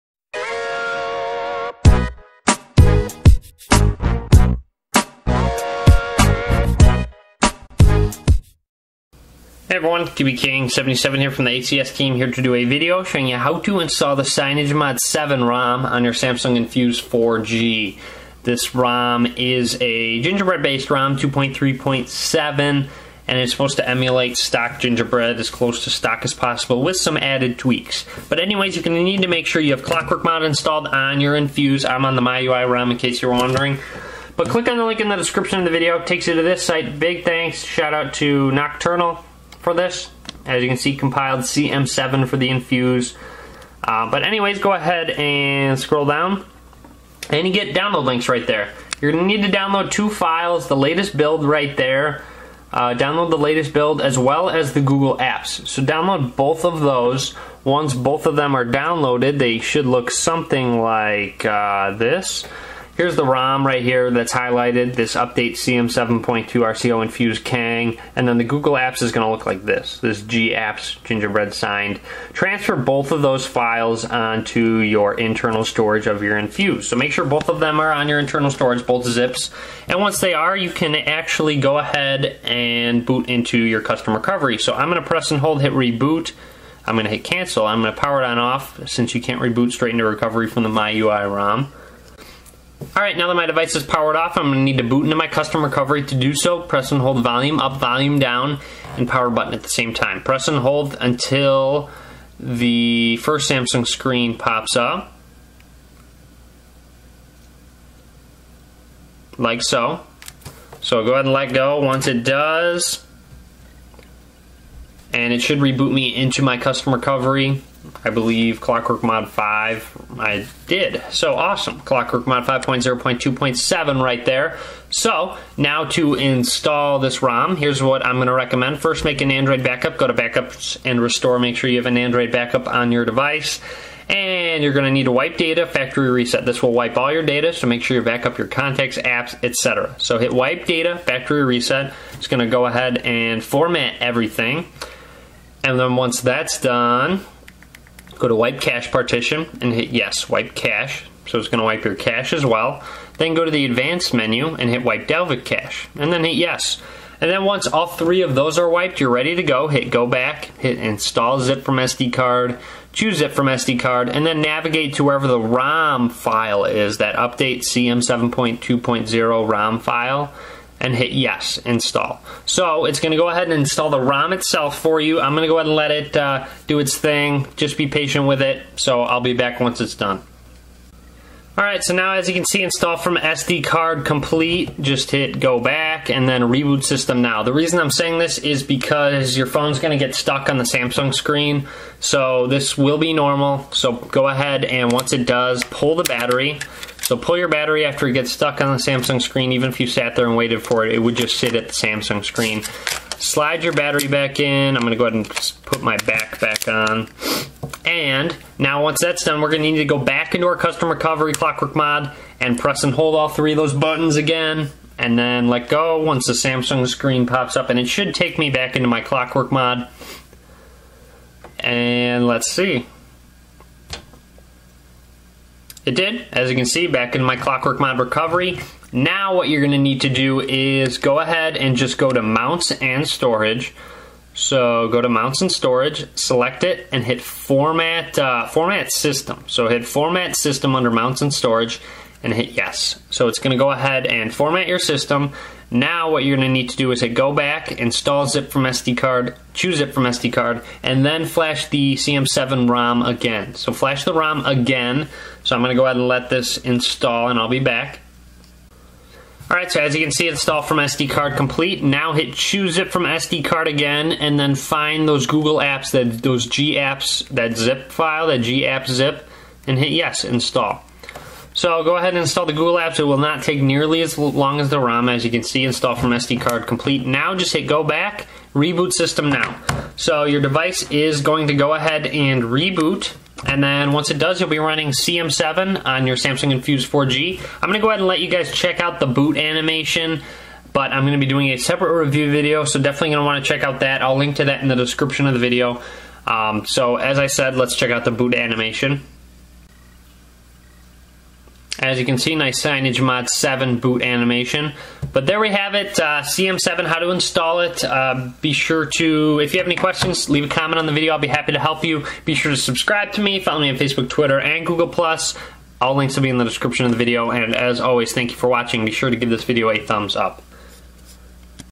How to Install CyanogenMod 7 Rom on the Samsung Infuse 4G
This video will show you how to install the CM7 rom on your Samsung Infuse 4G. Any questions, please ask.